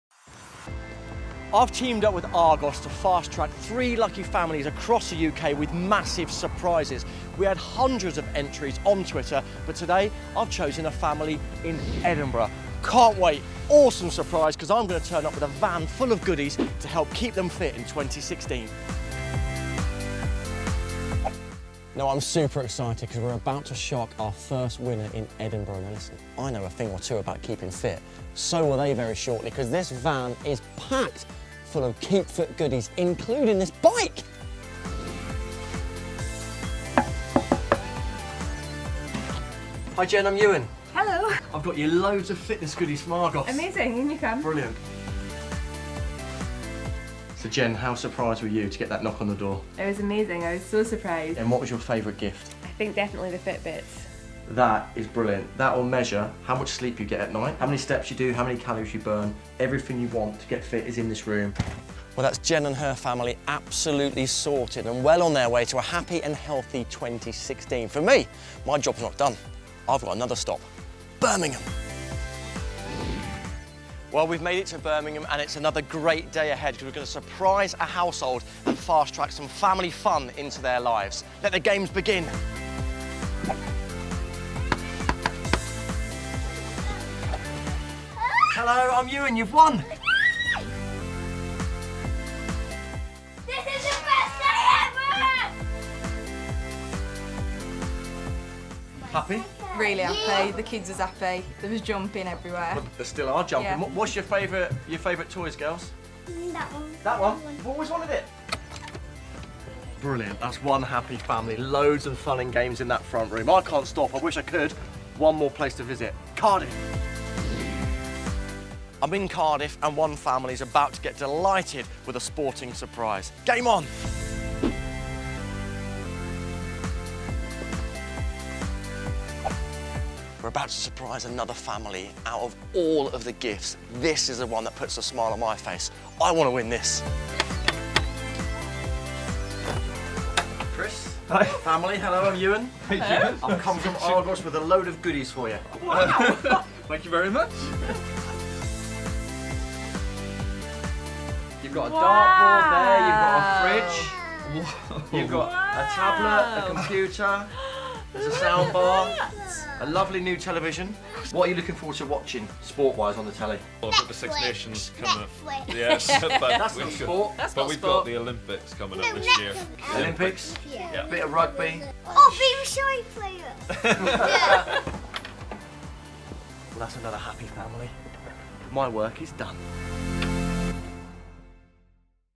Iwan Thomas surprises winning families across the UK
Watch our video as families who beat hundreds of other entries on Twitter get a special delivery from Iwan Thomas and the Argos Fast Track team as their homes are kitted out with the newest sporting equipment, family products and technology. Iwan pays a visit to families in Edinburgh, Cardiff and Birmingham.

For all this and more watch our video to see these lucky individuals reactions.